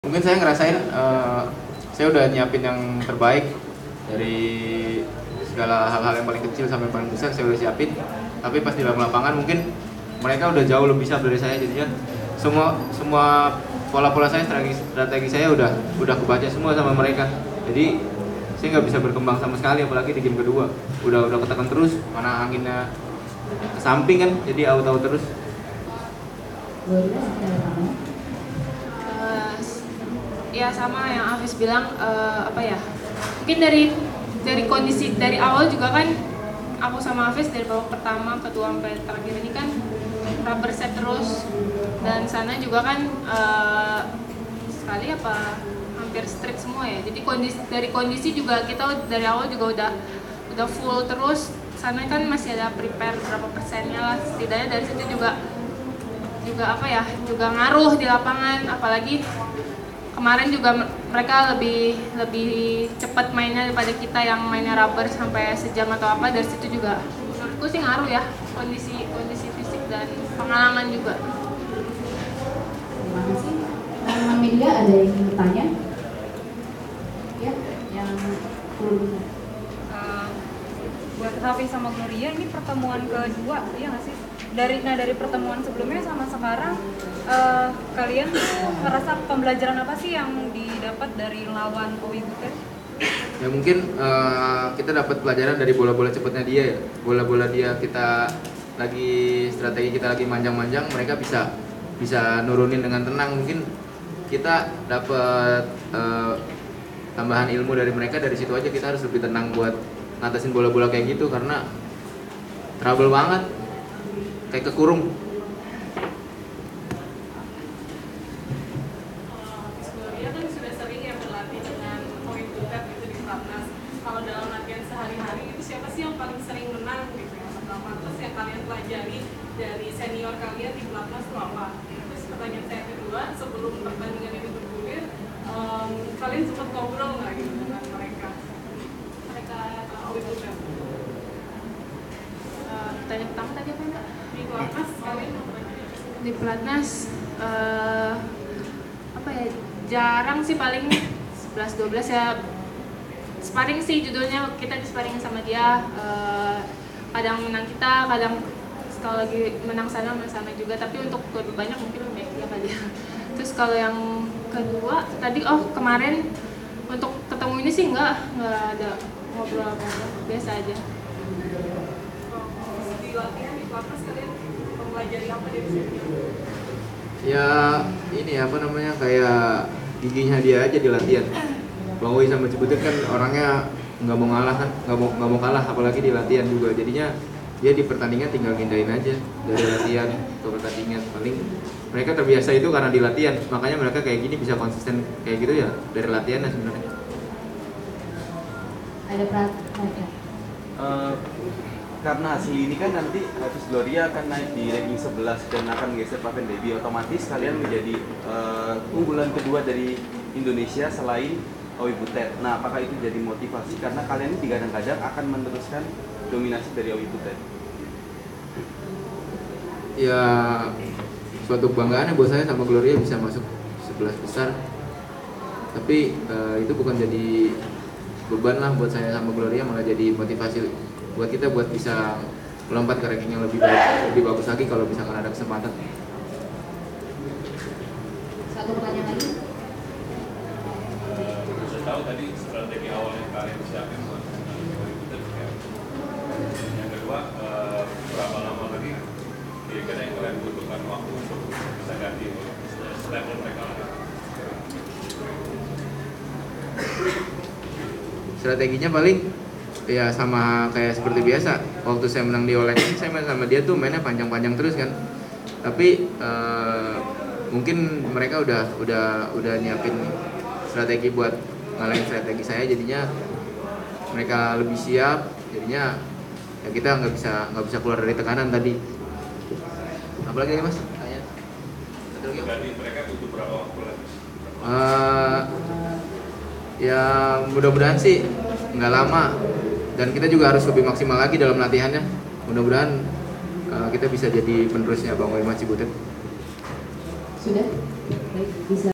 Hafiz/Gloria Mendapat Pelajaran dari Owi/Butet
Indonesia Open 2018 (Semifinal, 7 Juli 2018)